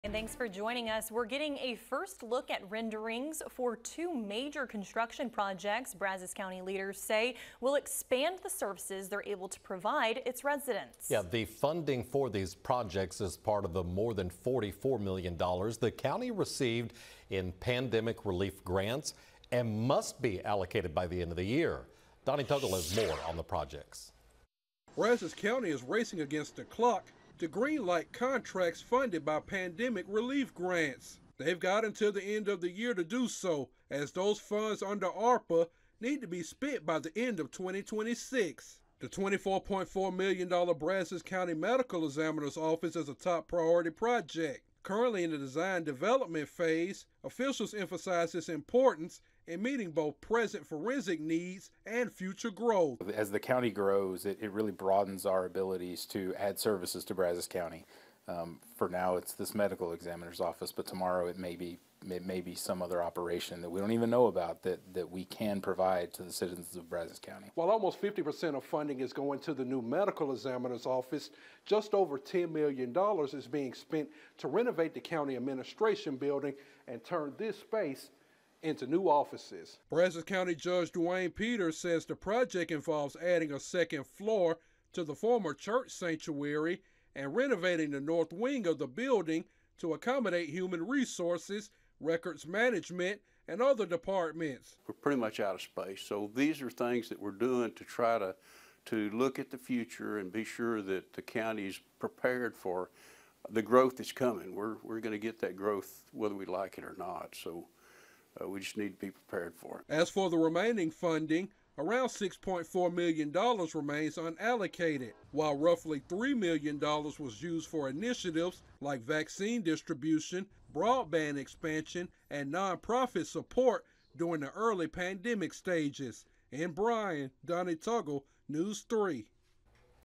Brazos County Officials Unveil Plans for New Medical Examiner's Office and Administrative Building
BRYAN, Texas (KBTX) -Time is winding down for Brazos County to designate and approve contracts that are being funded with pandemic relief grants. APRA funds must be allocated by the end of the year and spent by the end of 2026.

On Tuesday, commissioners received two presentations from county staff on the status of projects being funded with money from the American Rescue Plan Act (ARPA), projects county leaders say will expand the services they’re able to provide residents.

The new Brazos County Medical Examiner’s Office, estimated at $24.4 million, tops the list. With the design development phase underway, the county says the space will serve the forensic needs of the county while accommodating future growth.

“As the county grows it really broadens our abilities to add services to Brazos County. For now, it’s this medical examiner’s office, but tomorrow it may be some other operation that we don’t even know about that we can provide to the citizens of Brazos County,” said Brazos County Project Manager Trevor Lansdown.

While almost 50 percent of funding is going to the new medical examiner’s office, just over $10 million has been set aside to turn this space into more offices at the county administration building. This includes adding a second floor to the former church sanctuary and renovating the north wing of the building to make space for human resources, records management, and other departments.

“We’re pretty much out of space so these are things that we’re doing to try to look at the future and be sure that the county’s prepared for the growth that’s coming. We’re going to get that growth whether we like it or not,” said Brazos County Judge Duane Peters.

As for the remaining funding, around $6.4 million dollars are currently unobligated and another three million went to initiatives like vaccine distribution and nonprofits during the early stages of the pandemic.